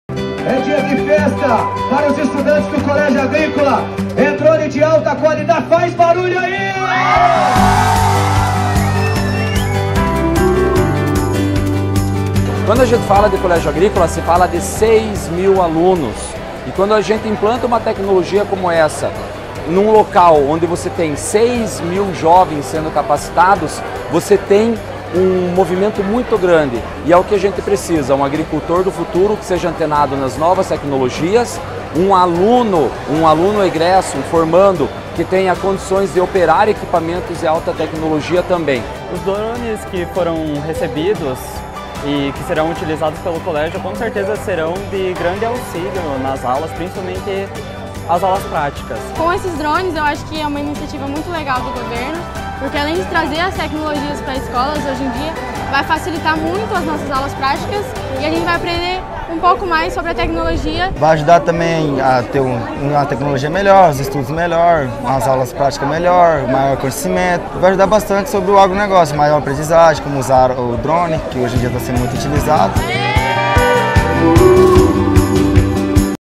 23 drones para os colégios agrícolas e florestais do Paraná
É mais tecnologia chegando para os colégios agrícolas e florestais do Paraná! 🌱🏫

Os drones, equipamentos altamente tecnológicos e necessários para a melhoria da produtividade, vão garantir aos estudantes acesso ao que há de mais moderno na agropecuária, garantindo, dessa forma, um futuro melhor para eles e para suas famílias.